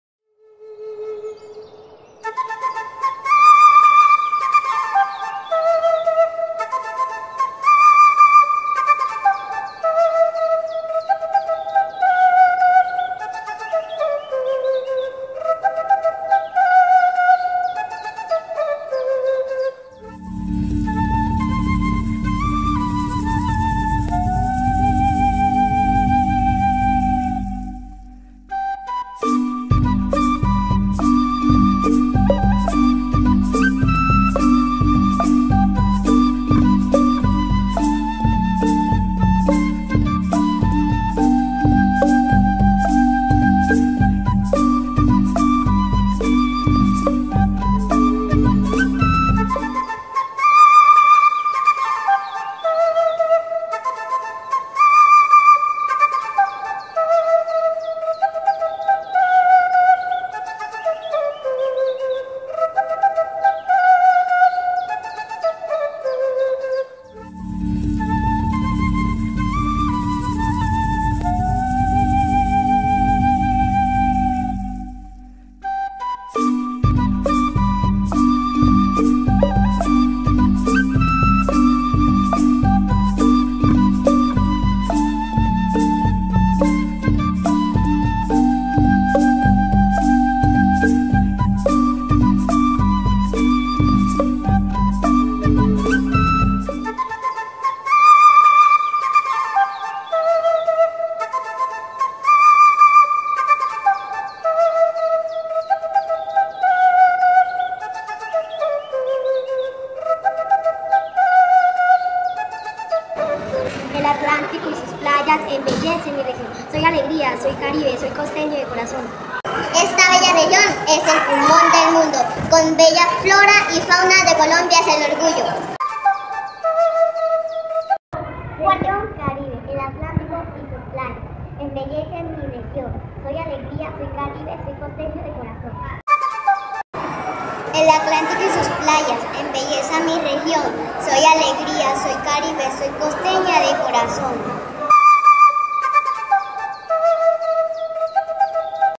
Rescatando la tradición oral.
El grado 1-2 sede A jornada Única con gran orgullo y regocijo compartimos el proceso y cierre de proyecto del segundo periodo: Rescatando la tradición oral.